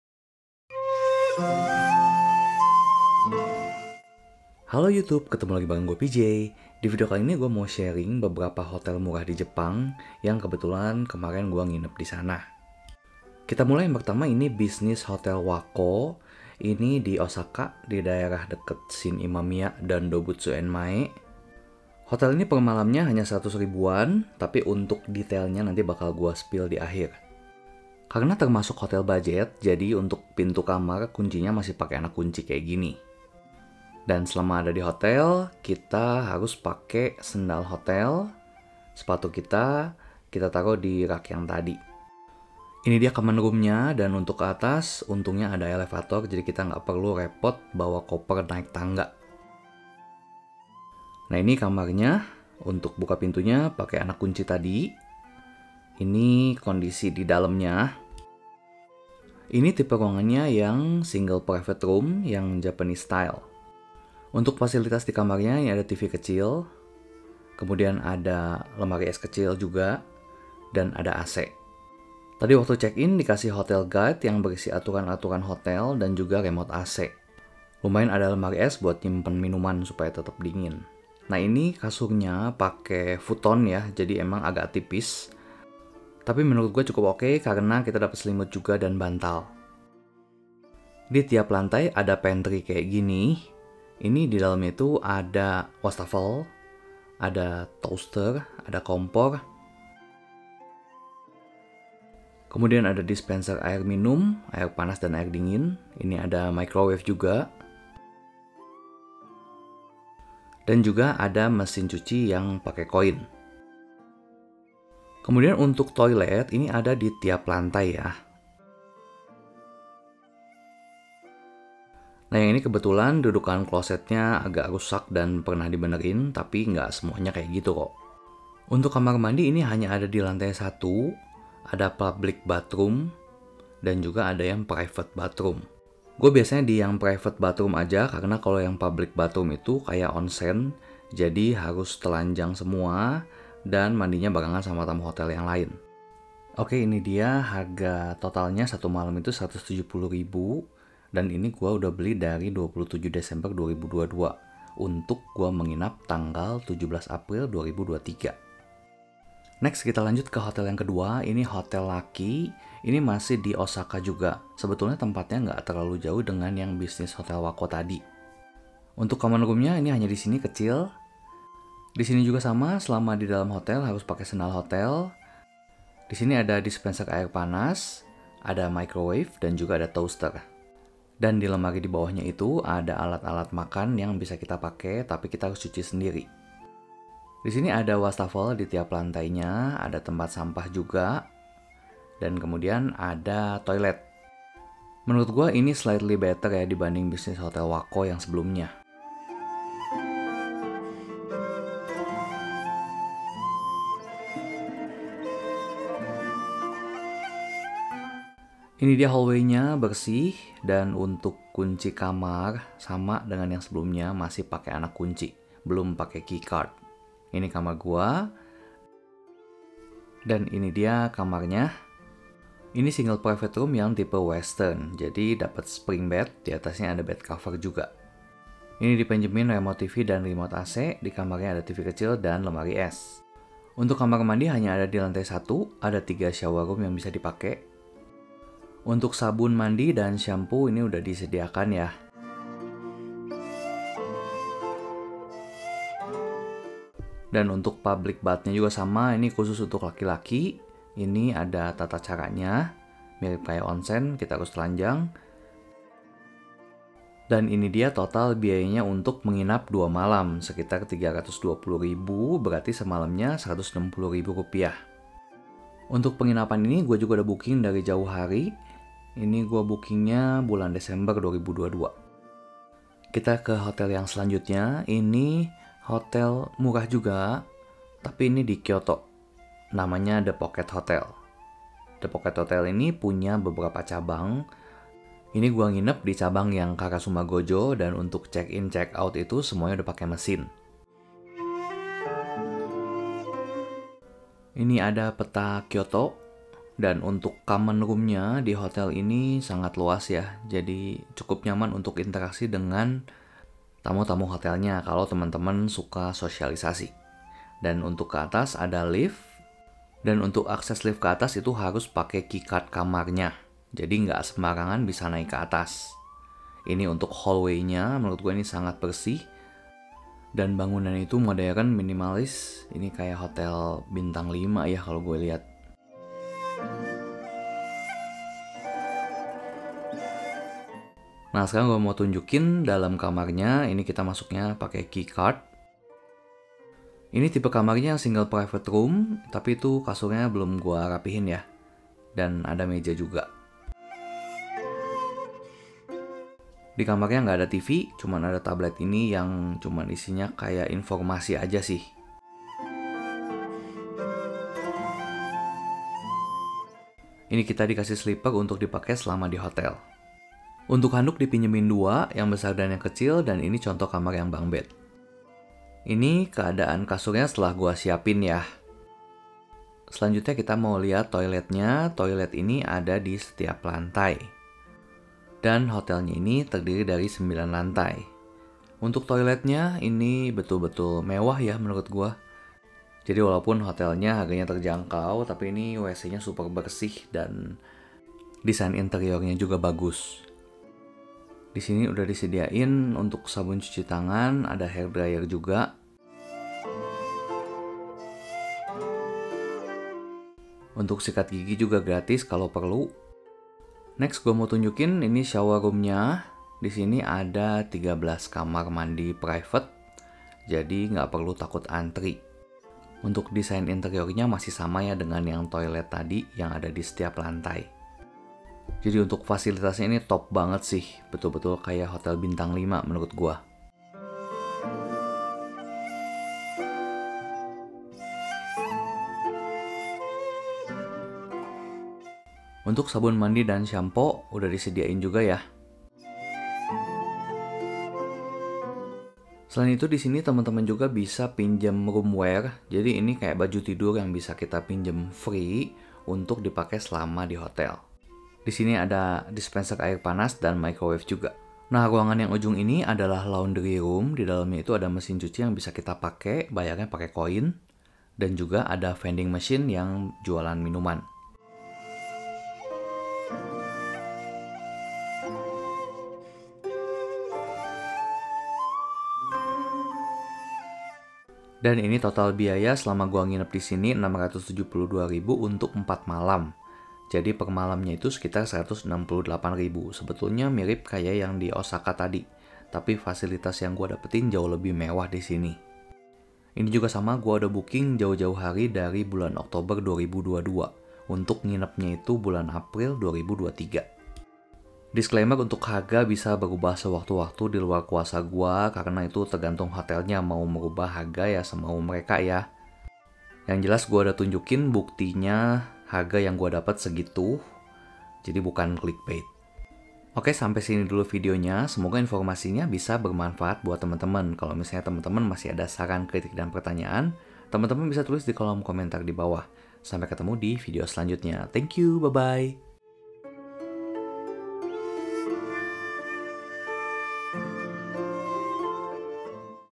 Hotel murah di Osaka dan Kyoto Jepang mulai dari 100 ribuan
Ke Jepang bisa hemat juga dengan menginap di hotel budget